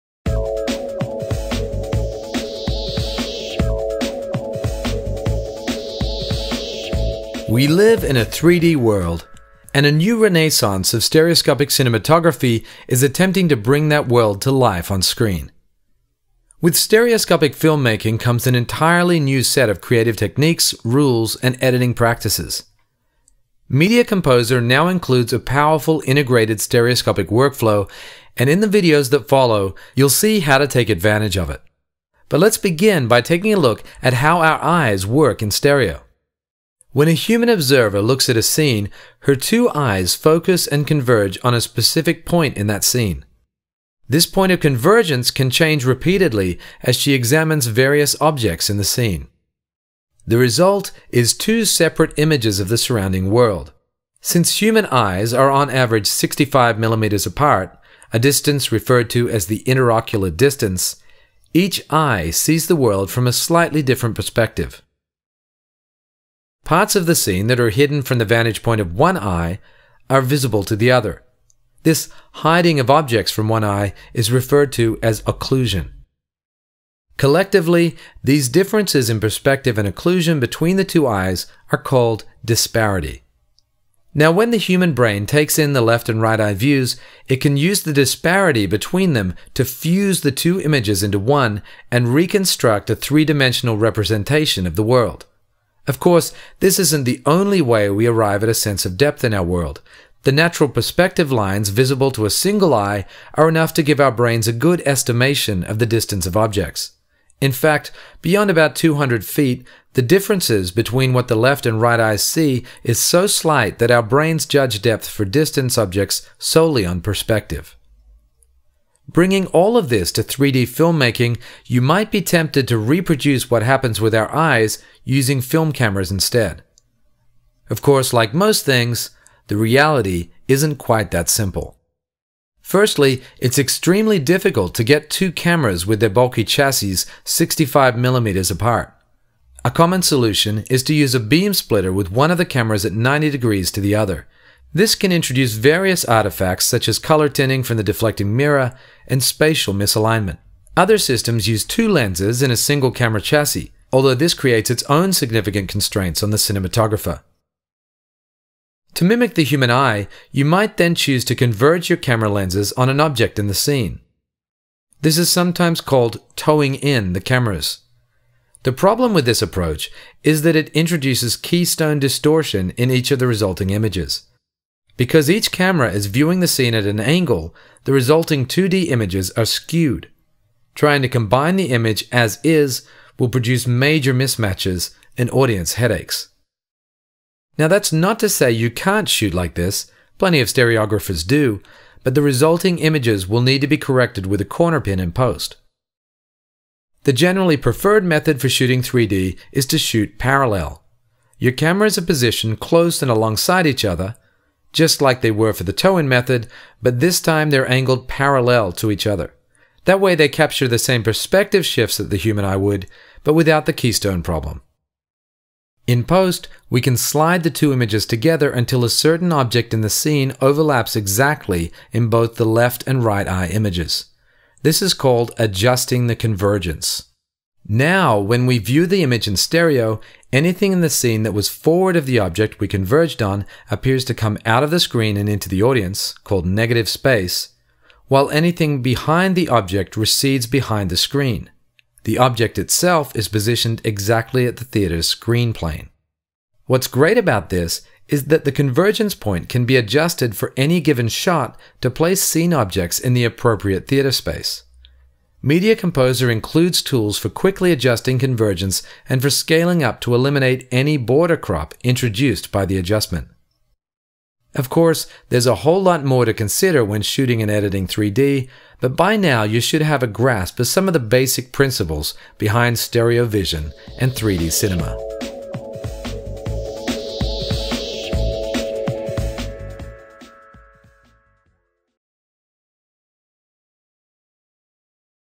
Stereoscopic Features: Introduction to Stereoscopic 3D
With Stereoscopic filmmaking comes an entirely new set of creative techniques, rules and editing practices. Media Composer 6 includes a powerful, integrated stereoscopic workflow. In our free tutorial videos on Avid.com you'll see how to take advantage of it. But, let's begin by taking a look at how our eyes work in stereo.